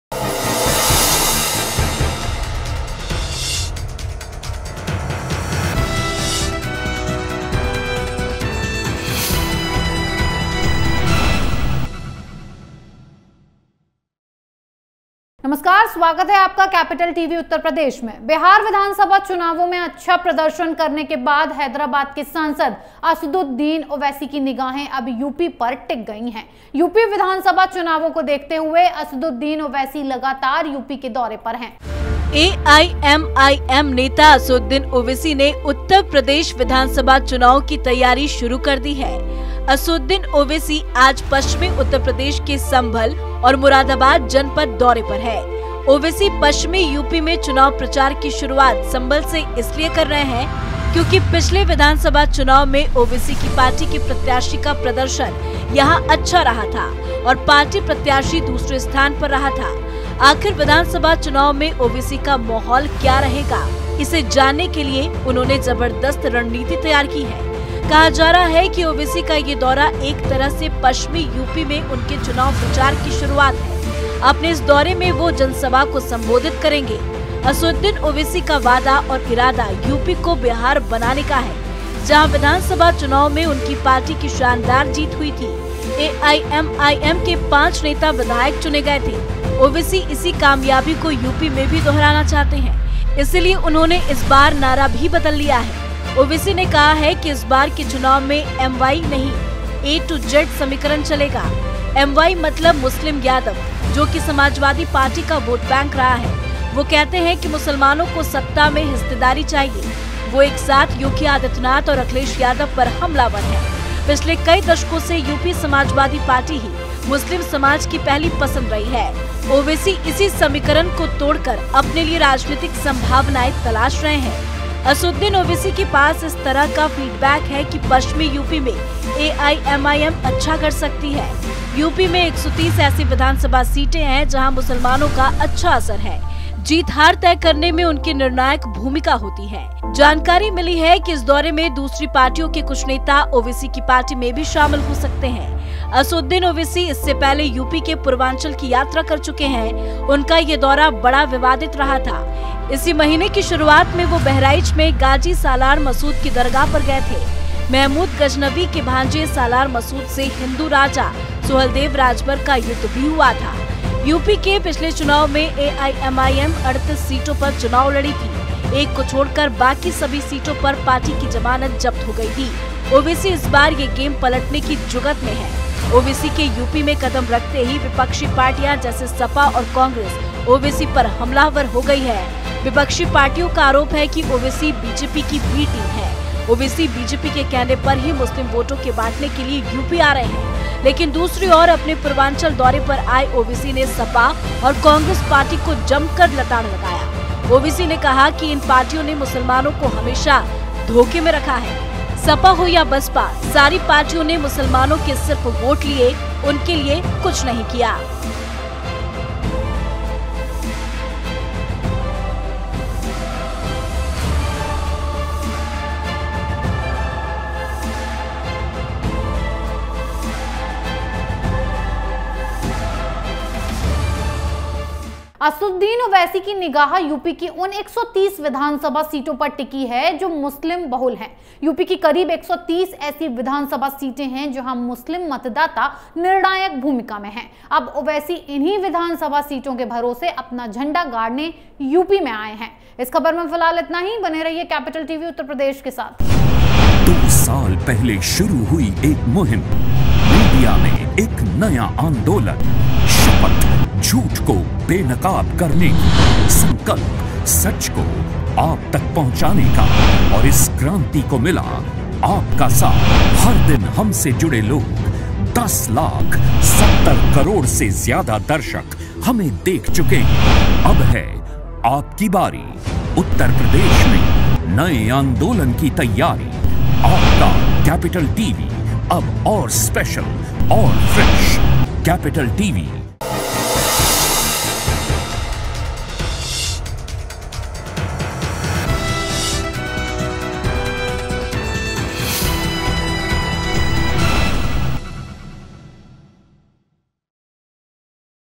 Asaduddin Owaisi Is On A Tour Of Western Uttar Pradesh | Capital TV Uttar Pradesh
AIMIM leader Asaduddin Owaisi has started preparations for the Uttar Pradesh Assembly Elections. Asaduddin Owaisi is on a tour of Sambhal and Moradabad districts of western Uttar Pradesh. Owaisi is starting the election campaign in western UP with a care because in the last assembly elections the performance of Owaisi's party's candidate was good here and the party candidate stood second. After all, what will be the atmosphere of Owaisi in the assembly elections. To know this, he has devised a tremendous strategy.